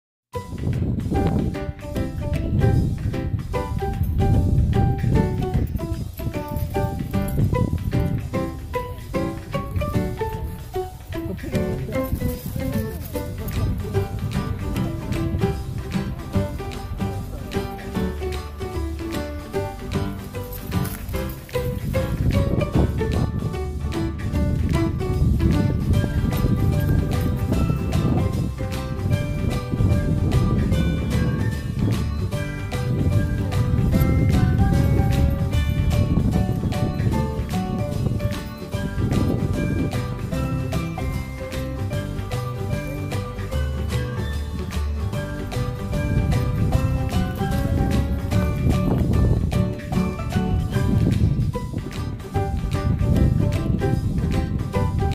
Playdate Mayhem
Tate, Bess and Boomer playing at Riverside